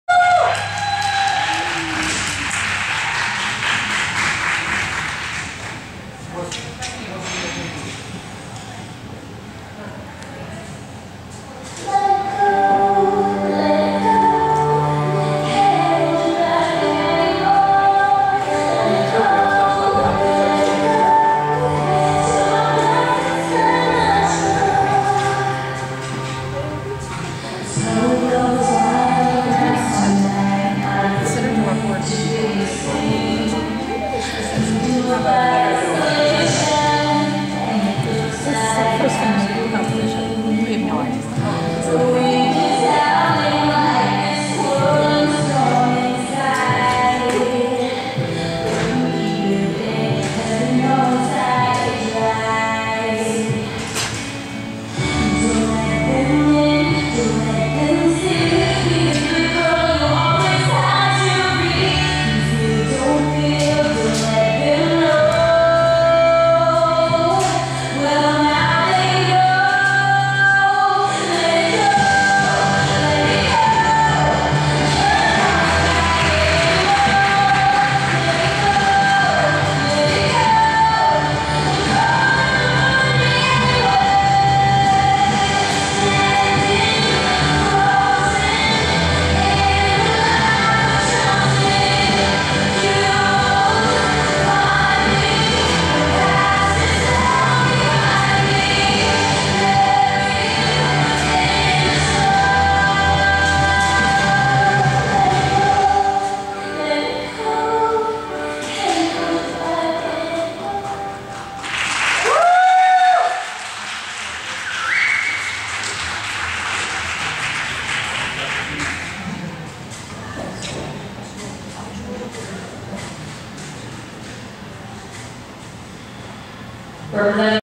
Emilys competition at Skate Zone May 2014
Emily's program at Skate Zone, May 2014.  We are so proud of all her hard work and her courage at putting herself out on the ice by herself to show what she has learned.  Here's what hours of practice can accomplish when you have a true love of a sport.  Everyone skated beautifully and we are so proud of all of our Team Liberty friends, your all 1st place with us!